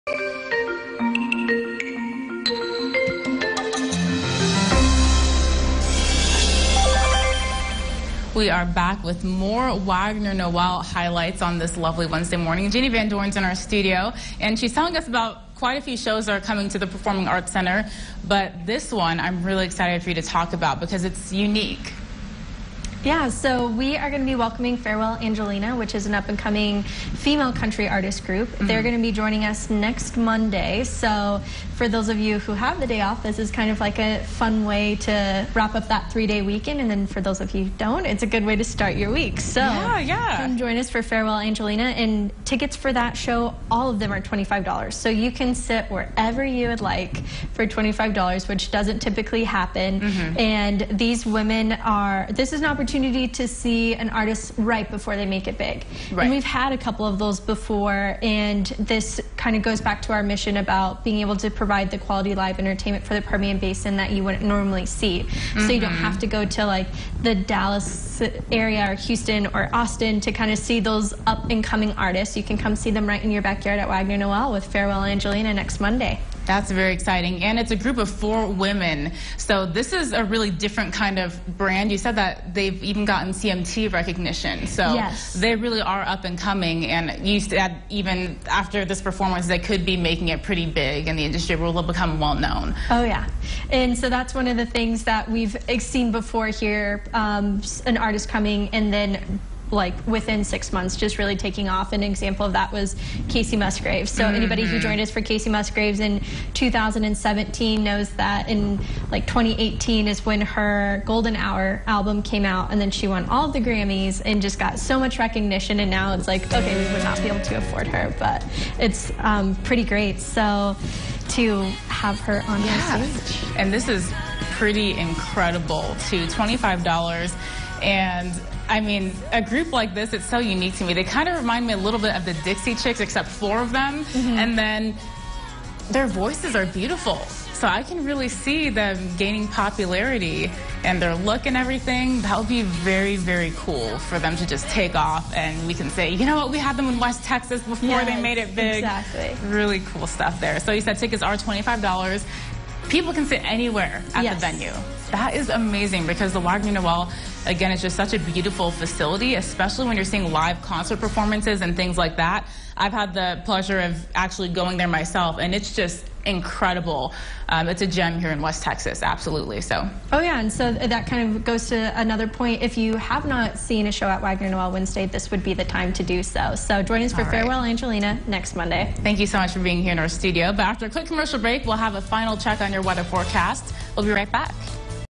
Wagner Noël Wednesday part 2
Ginny Van Doren stopped by the studio this morning to tell us about some exciting shows coming up at the Wagner Noël Performing Arts Center!